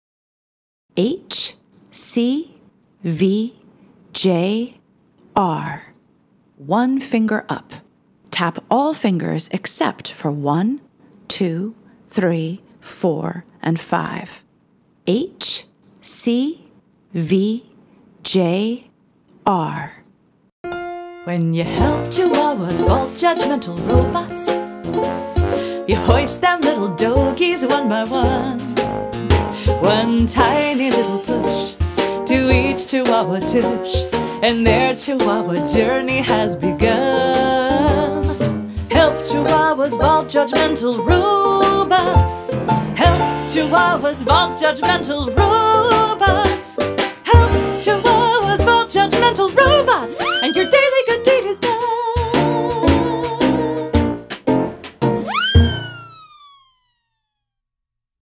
Chapter 5: HCVJR - One Finger Up (Left Hand)
Learn how to TAP the letters HCVJR with your left hand.